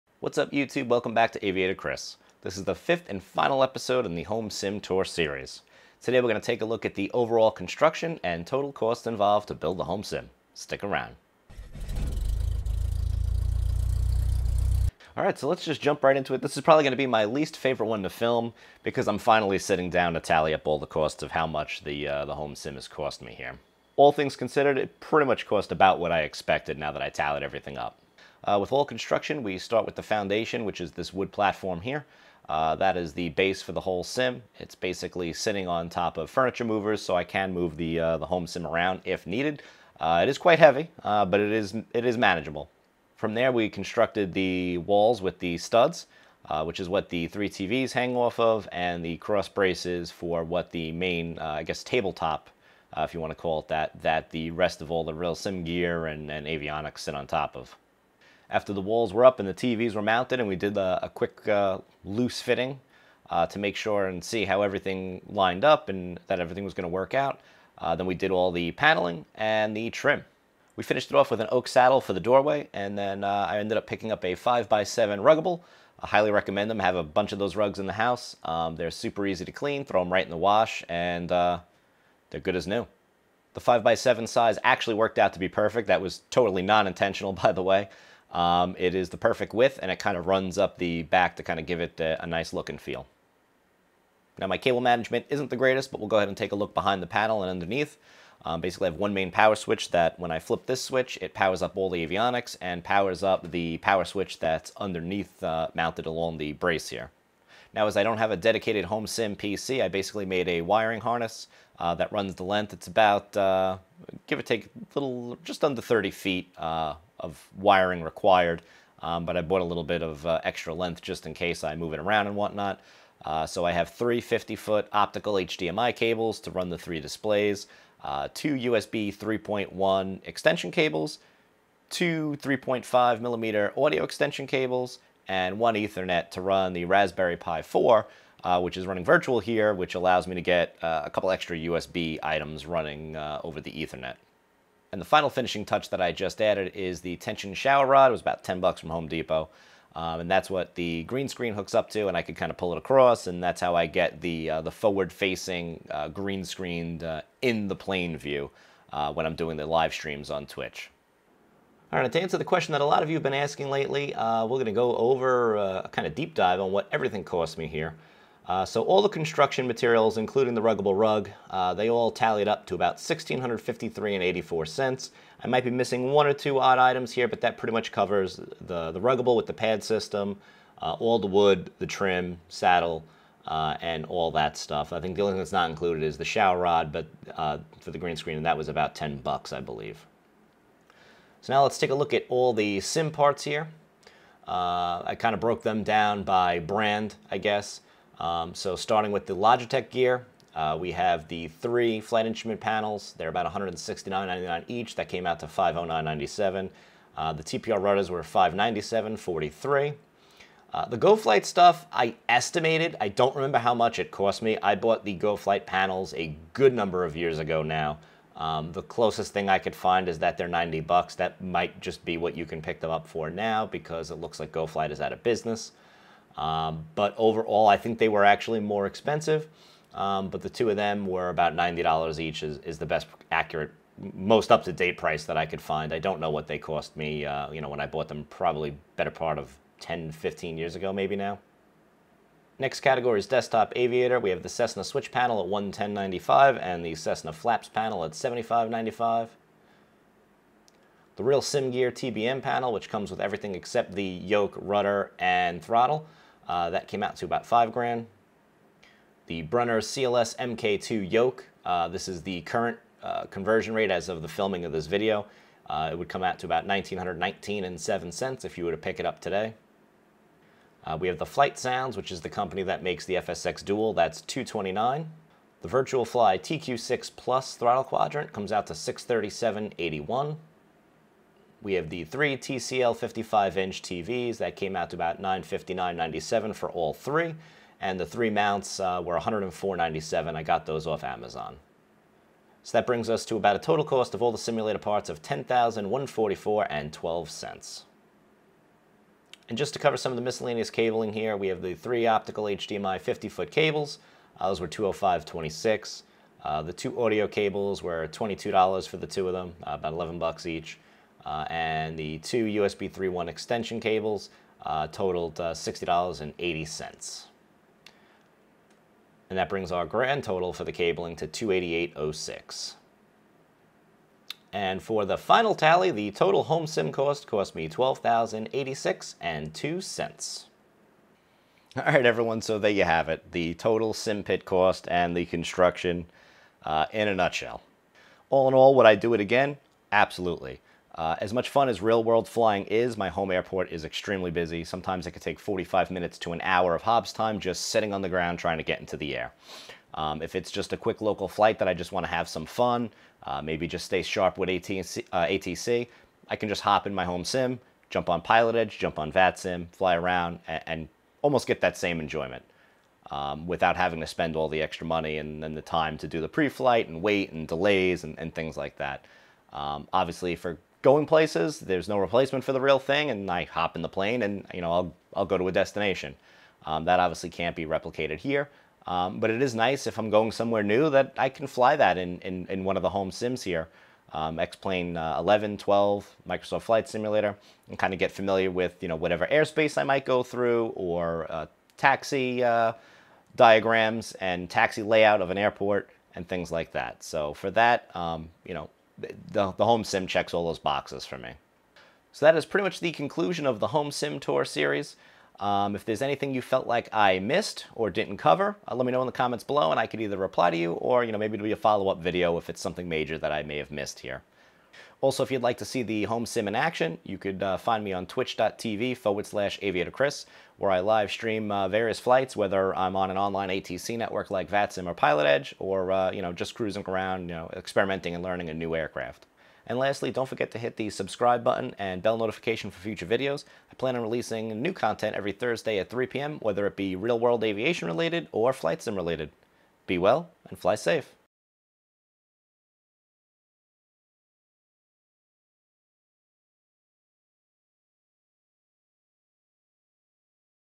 Home Flight Simulator Tour Series - Construction and Final Costs - Episode 5
TCL 55" TVs (Below is updated model)

In this episode we take a look at the construction of the home simulator and tally up the final costs!

0:00 Intro
0:14 Jumping in
0:27 Construction
1:35 Cable Management
2:41 Cost Breakdown
5:31 Real world vs Home Sim
6:41 Conclusion